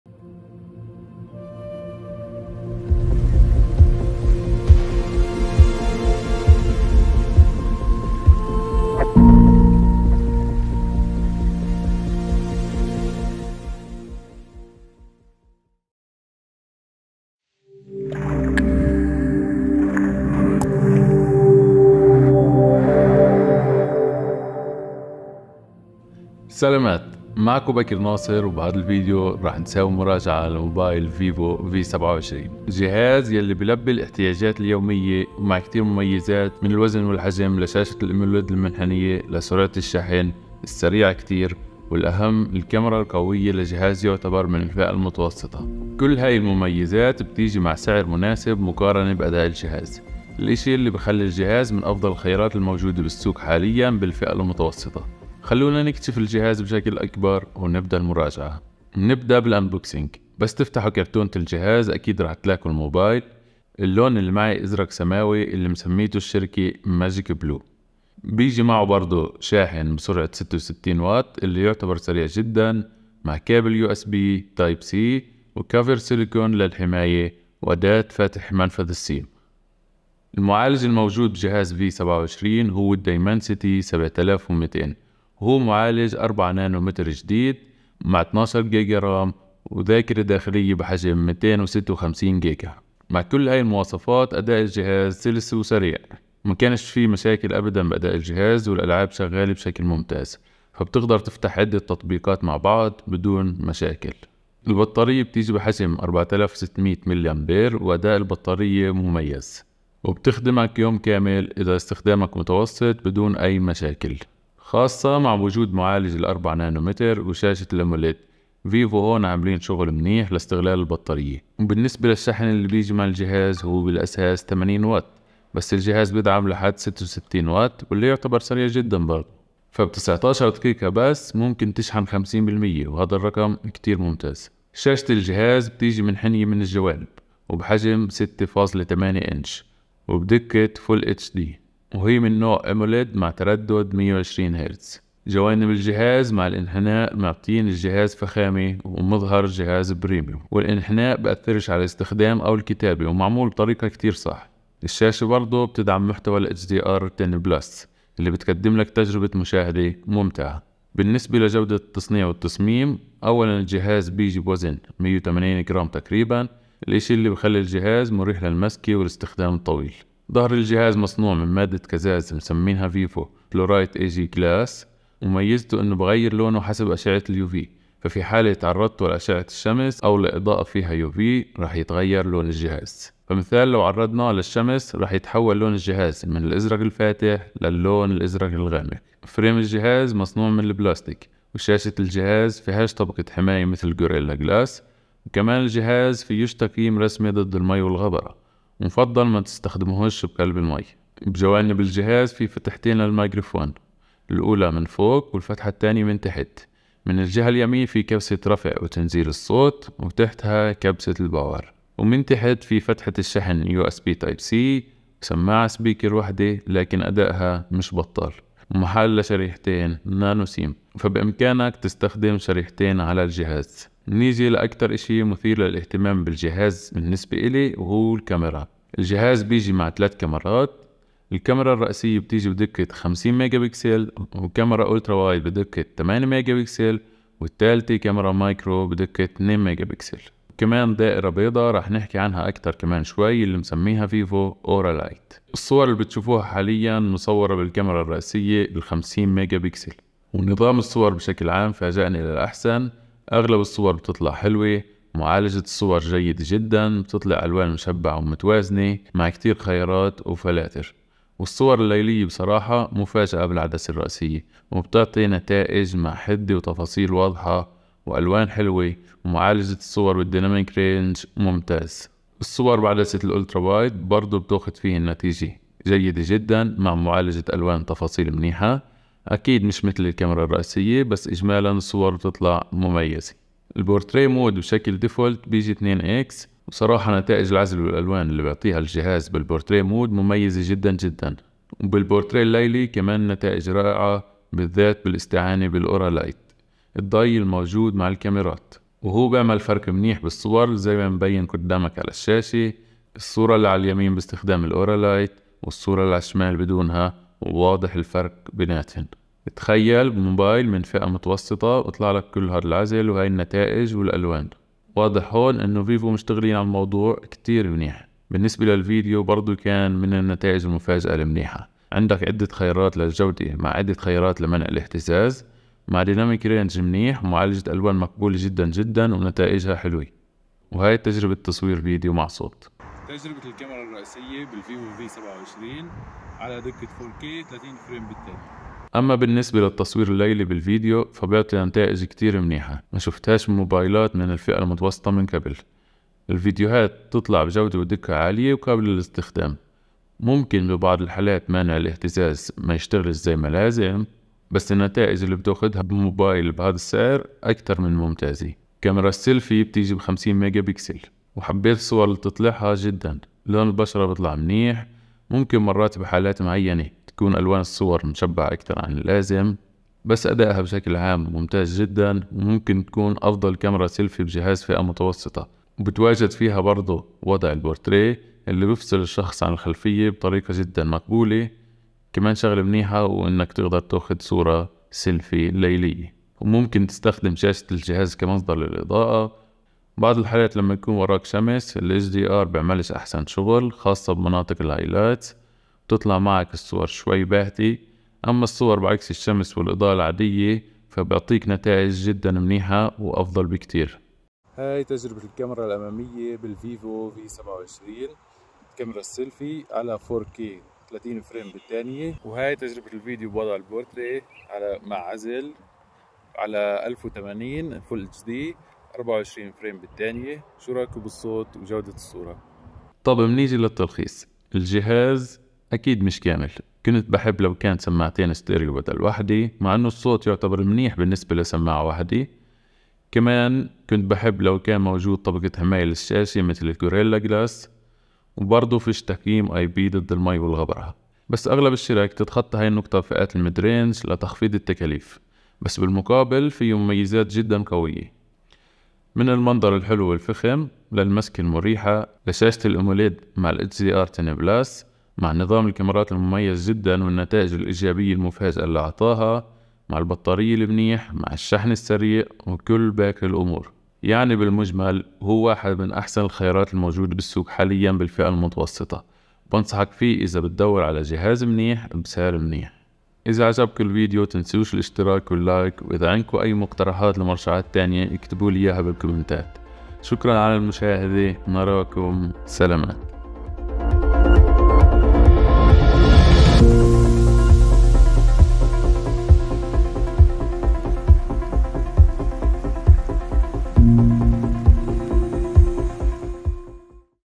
موبايل رخيص وكاميرات قوية؟ مراجعة فيفو VIVO V27
في هذا الفيديو سوف أعرض مراجعة لجوال فيفو في27، من افضل هواتف سنة 2023 في الفئة المتوسطة.

محتوى الفيديو:
00:00 تشويقة
00:18 المقدمة
00:59 انبوكسنج
01:20 المواصفات
01:45 البطارية والشحن
02:15 الشاشة
02:41 التصميم وتغيير اللون
03:44 الكاميرات
07:03 الخلاصة